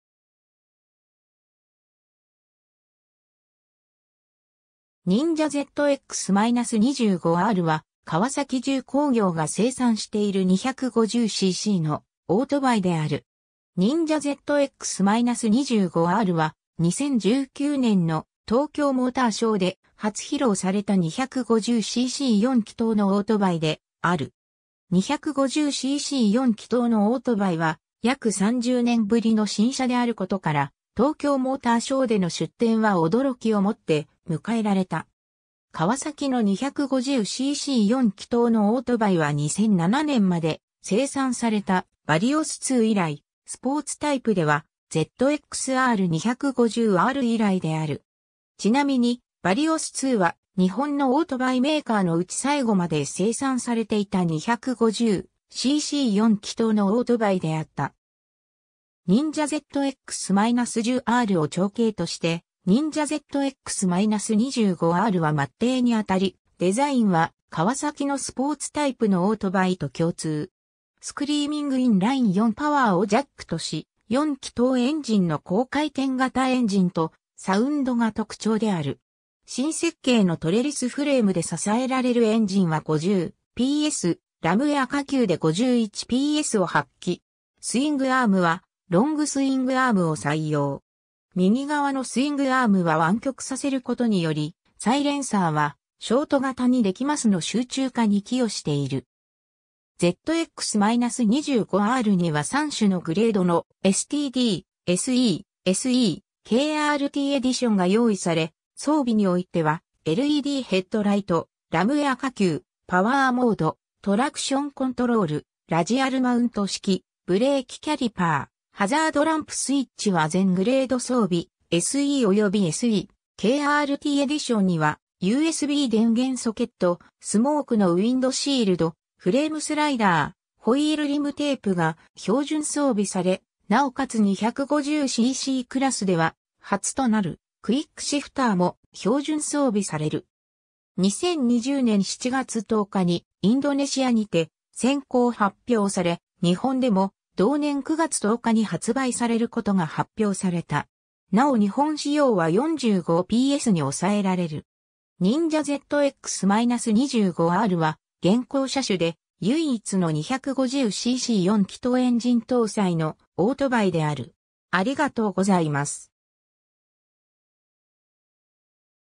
カワサキ・ニンジャZX-25R
ニンジャZX-25R (Ninja ZX-25R ニンジャゼットエックス・トゥエンティファイブ・アール)は川崎重工業(川崎重工業モーターサイクル&エンジンカンパニー)が生産している250 ccのオートバイである。
 ニンジャZX-25Rは、2019年の東京モーターショーで初披露された250 cc 4気筒のオートバイである。
250 cc 4気筒のオートバイは約30年ぶりの新車であることから、東京モーターショーでの出展は驚きをもって迎えられた。
カワサキの250 cc 4気筒のオートバイは2007年まで生産されたバリオスII以来、スポーツタイプではZXR250R以来である。
ちなみにバリオスIIは日本のオートバイメーカーのうち最後まで生産されていた250 cc 4気筒のオートバイであった。
 ニンジャZX-10Rを長兄としてニンジャZX-25Rは末弟にあたり、デザインはカワサキのスポーツタイプのオートバイと共通。
「SCREAMING IN-LINE 4 POWER」を惹句とし、4気筒エンジンの高回転型エンジンとサウンドが特徴である。
 新設計のトレリスフレームで支えられるエンジンは50 PS、ラムエア過給で51 PSを発揮。
スイングアームはロングスイングアームを採用。
右側のスイングアームは湾曲させることによりサイレンサーはショート型にできマスの集中化に寄与している。
ZX-25Rには3種のグレードのSTD (スタンダード)、SE (スペシャルエディション)、SE KRT EDITION (スペシャルエディション・カワサキレーシングチーム・エディション)が用意され、装備においてはLEDヘッドライト、ラムエア過給、パワーモード、トラクションコントロール、ラジアルマウント式ブレーキキャリパー、ハザードランプスイッチは全グレード装備、SEおよびSE KRT EDITIONにはUSB電源ソケット、スモークのウインドシールド、フレームスライダー、ホイールリムテープが標準装備され、なおかつ250 ccクラスでは初となるクイックシフターも標準装備される（STD仕様はすべてオプション）。
 2020年7月10日にインドネシアにて先行発表され、日本でも同年9月10日に発売されることが発表された。
なお日本仕様は45 PSに抑えられる(ラムエア加圧時は46 PS)。
ニンジャZX-25Rは現行車種で唯一の250 cc 4気筒エンジン搭載のオートバイである。